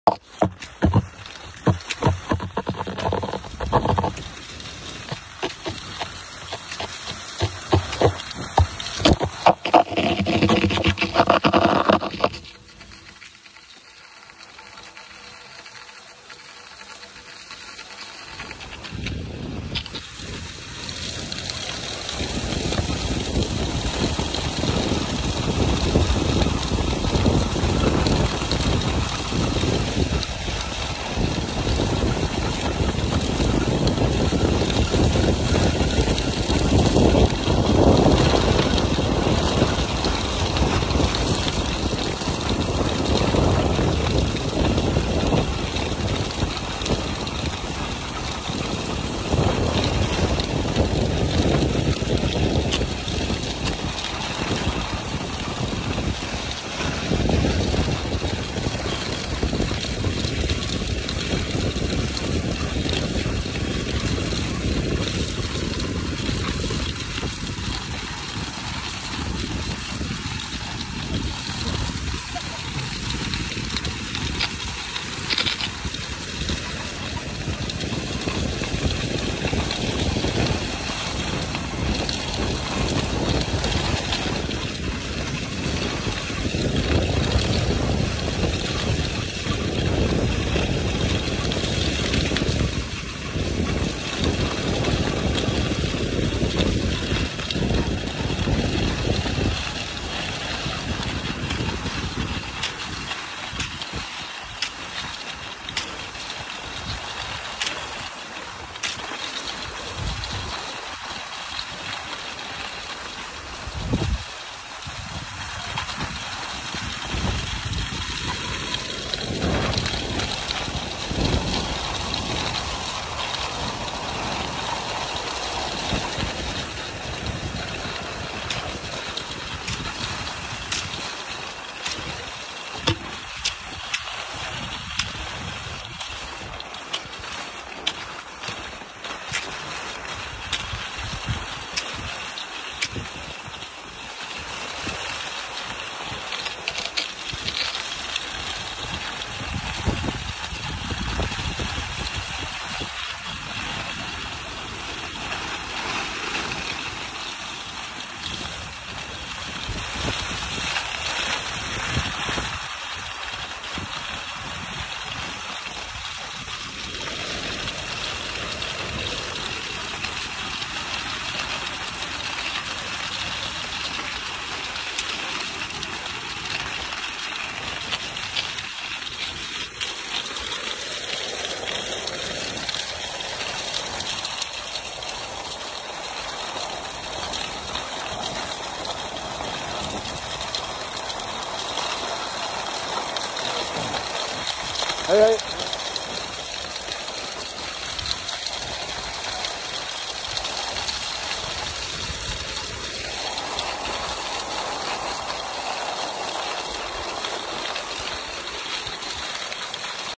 Geitryggen-Bånsætra vinter
Filmen viser løypetraseen fra Geitryggen forbi Demmingan og avsluttes ved Årnesfeltet på Bånsetra.